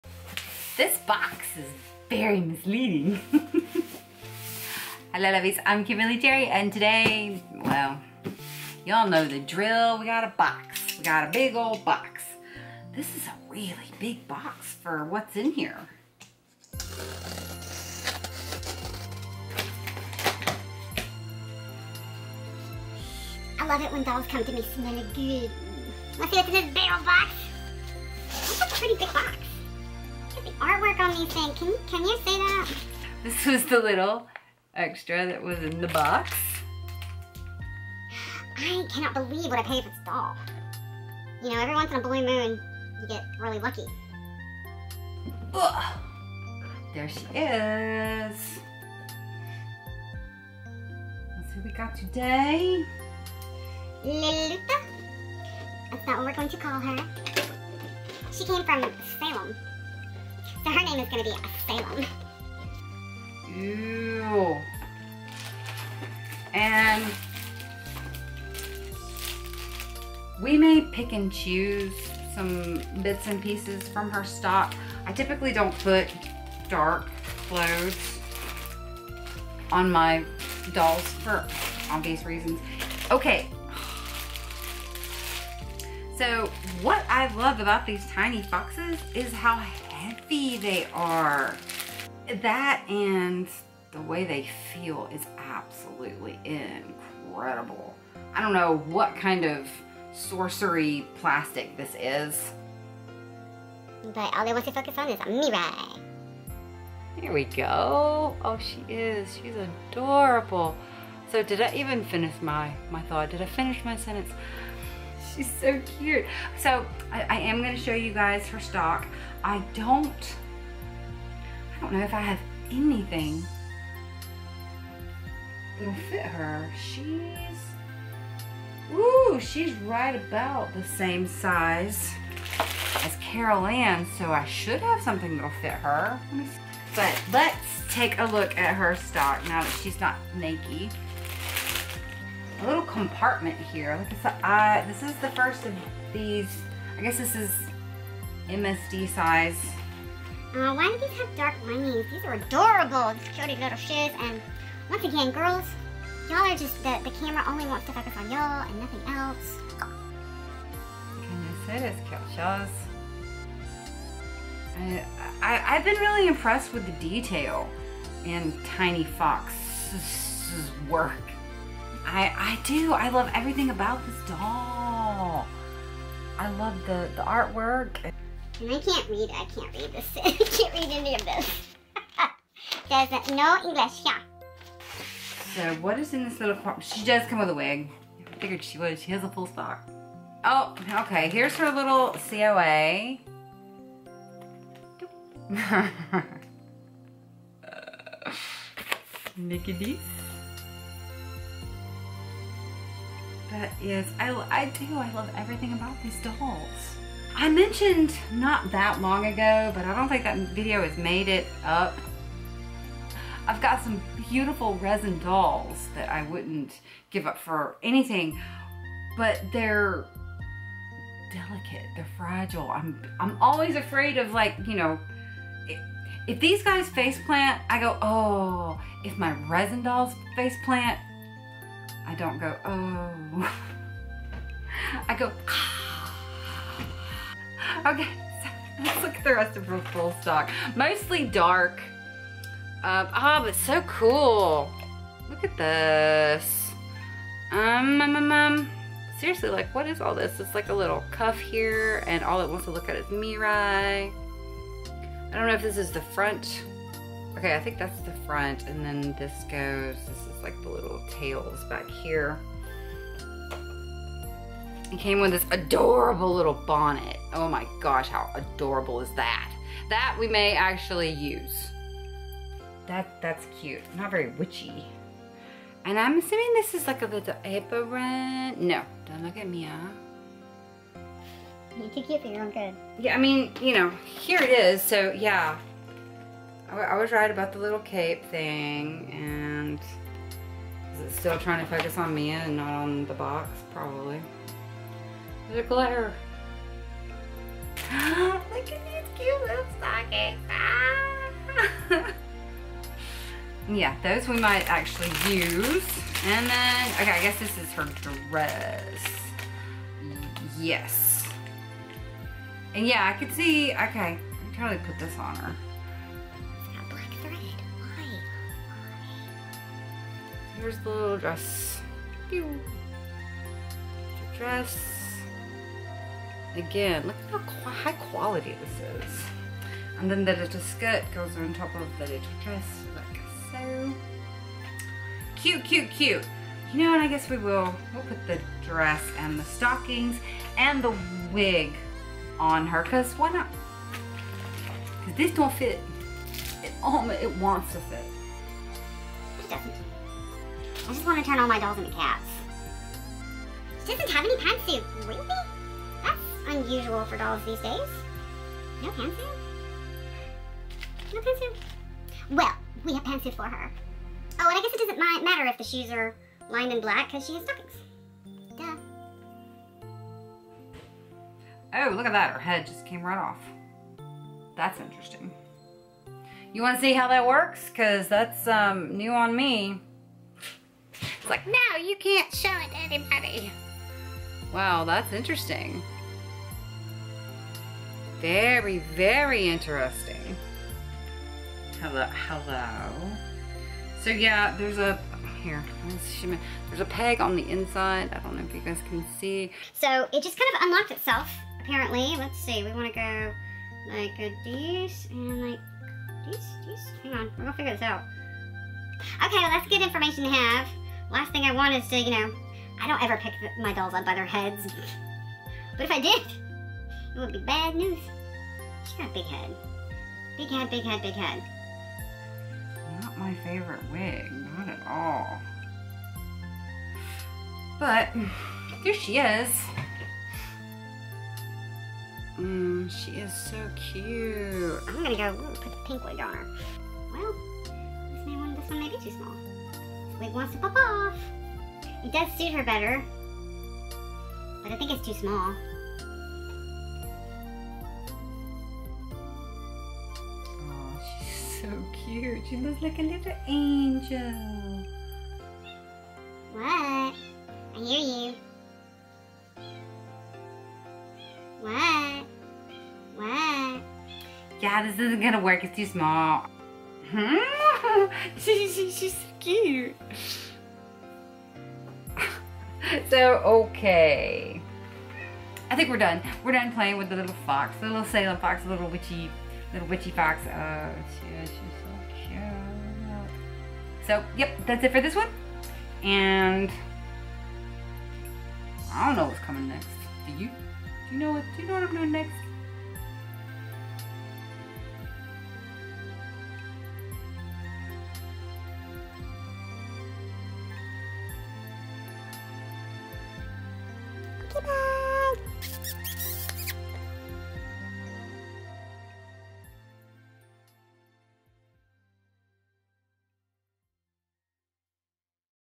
TinyFox Lulita BJD Doll Box Opening
a tinyfox doll opening

music from OCRemix